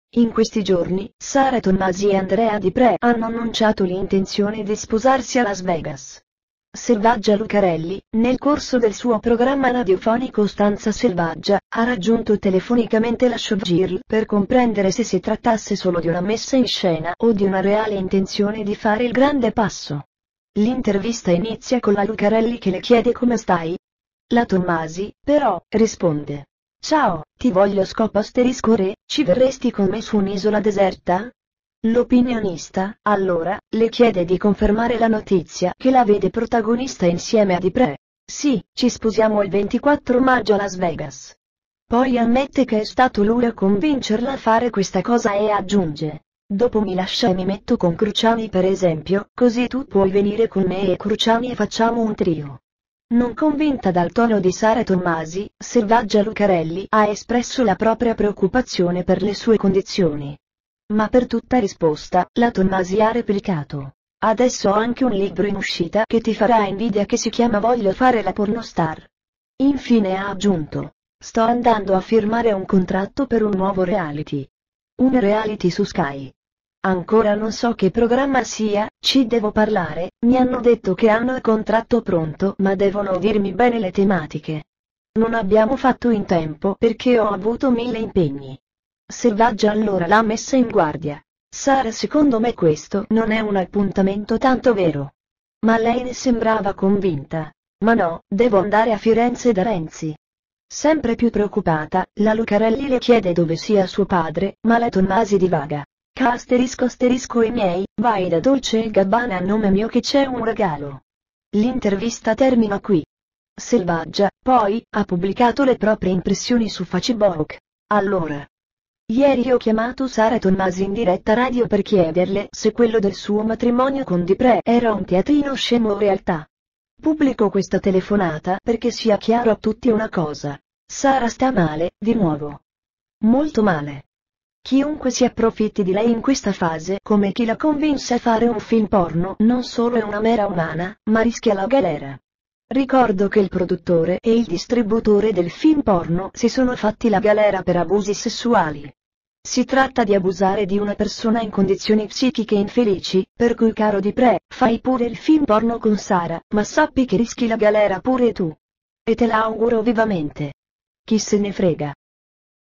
Sara Tommasi shock al telefono con la Lucarelli “Ti voglio scopare”
Selvaggia Lucarelli ha intervistato Sara Tommasi per verificare se la notizia del matrimonio con Andrea Diprè fosse vera. La showgirl, però, l'ha accolta esclamando: "Ciao, ti voglio scopare, ci verresti con me su un'isola deserta?".